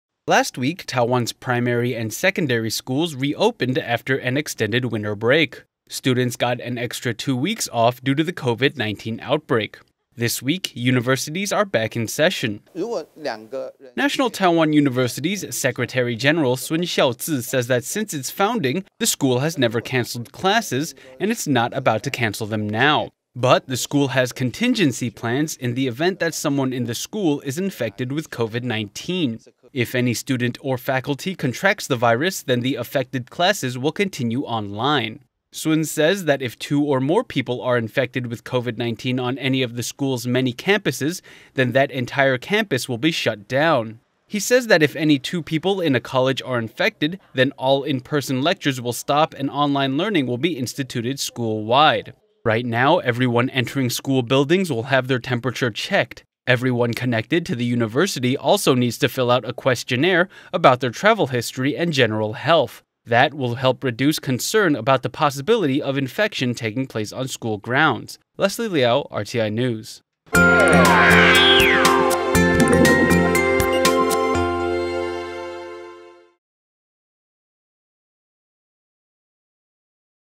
Classes begin at Taiwan’s universities; COVID-19 measures in place | Taiwan News | RTI
Taiwan’s university students headed back to class on Monday for the new semester. But authorities are taking special precautions to keep the virus at bay.